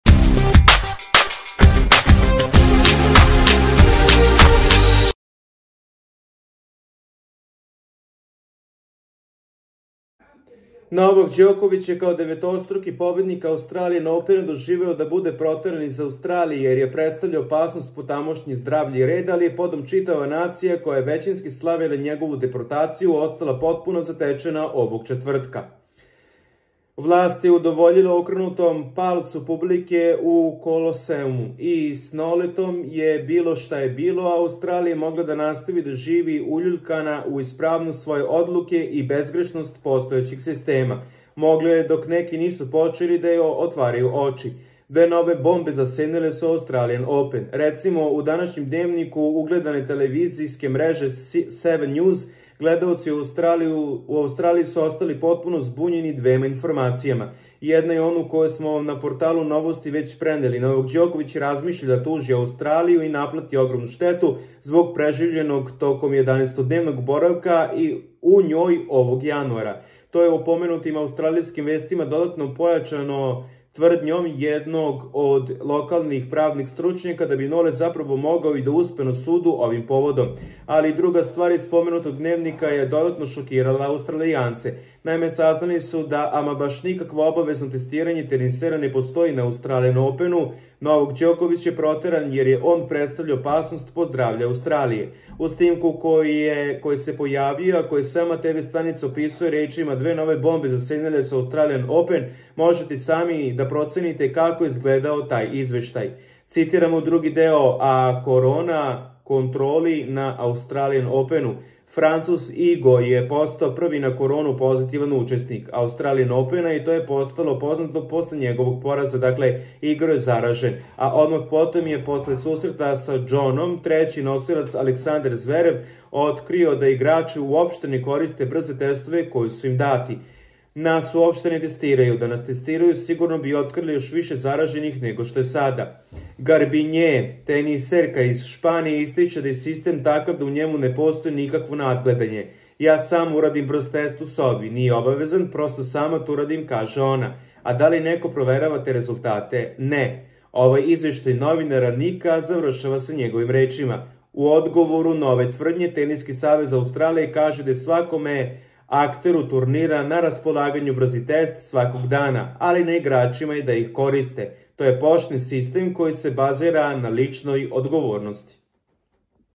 AUSTRALIJA GLEDA DNEVNIK U NEVERICI: Dve vesti šokirale naciju
NOVAK Đoković je, kao devetostruki pobednik Australijan opena, doživeo da bude proteran iz Australije jer je "predstavljao opasnost po tamošnje zdravlje i red", ali je potom čitava nacija, koja je većinski slavila njegovu deportaciju, ostala potpuno zatečena ovog četvrtka.